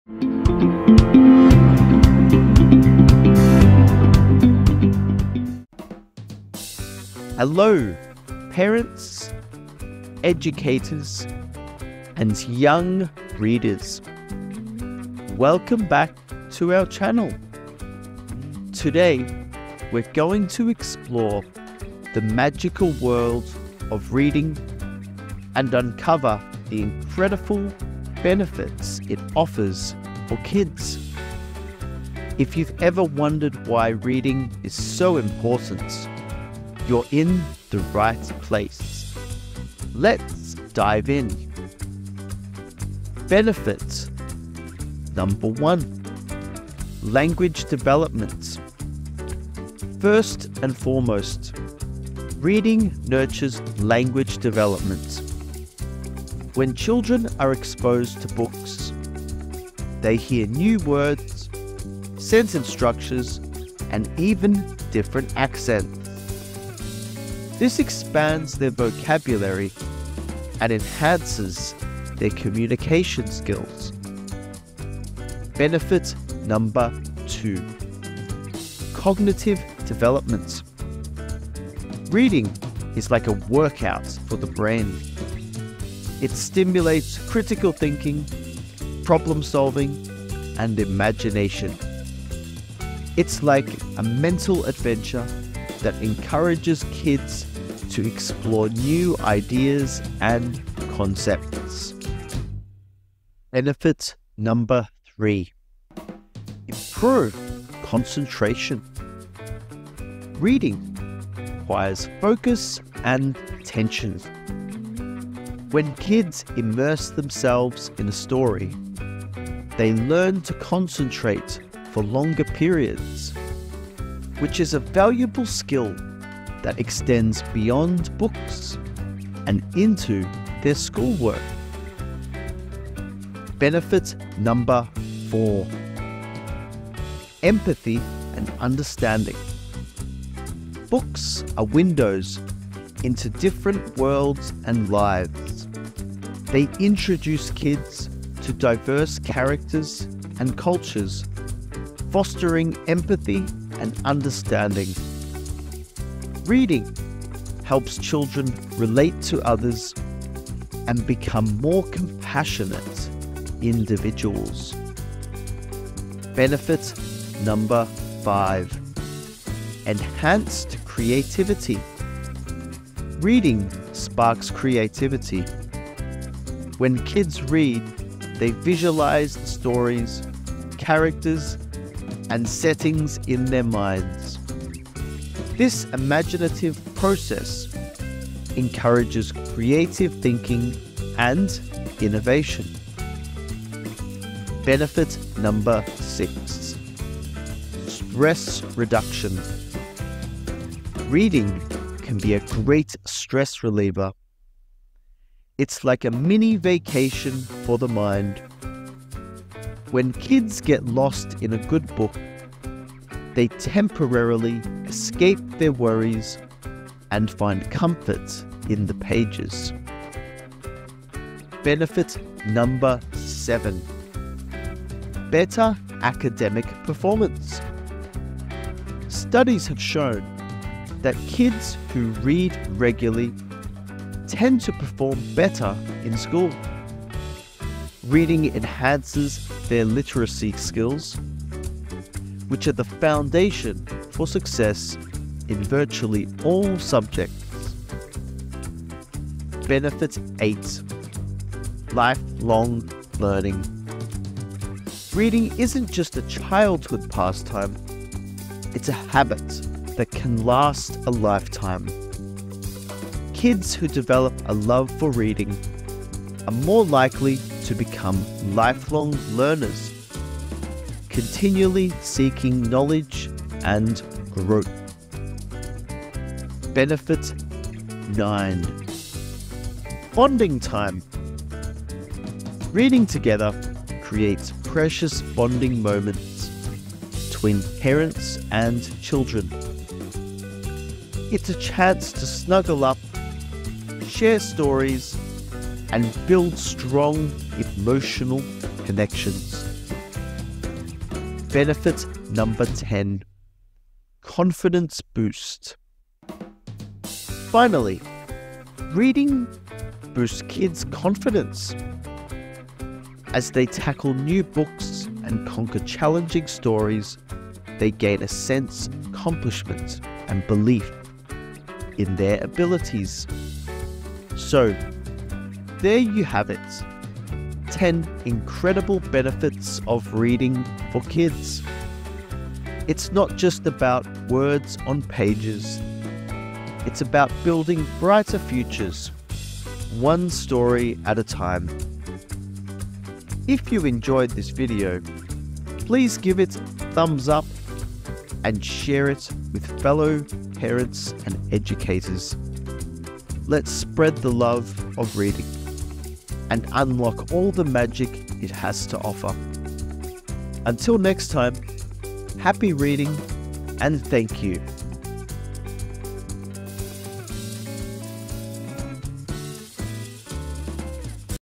How to Learn English | The Incredible Benefits of Reading for Kids
Learn English with Fun & Engaging Stories! | The Incredible Benefits of Reading for Kids

🌟 Join us on a journey to master the English language! In this video, we share top tips and strategies to improve your English skills. Whether you're a beginner or looking to enhance your fluency, our video is packed with valuable insights.

📖 What You'll Learn:

Pronunciation Techniques
Speaking Practice Methods
Fun Reading Activities
Enhancing Reading Comprehension
Utilizing Online Resources
Joining Book Clubs and Language Groups
The Importance of Asking Questions
💬 Feedback & Free Resources: We'd love to hear your thoughts and answer any questions!

TIMESTAMPS
00:00 Introduction
00:43 [Benefit 1: Language Development]
01:15 [Benefit 2: Cognitive Development]
01:44 [Benefit 3: Improved Concentration]
02:13 [Benefit 4: Empathy and Understanding]
02:45 [Benefit 5: Enhanced Creativity]
03:13 [Benefit 6: Stress Reduction]
03:42 [Benefit 7: Better Academic Performance]
04:09 [Benefit 8: Lifelong Learning]
04:37 [Benefit 9: Bonding Time]
05:01 [Benefit 10: Confidence Boost]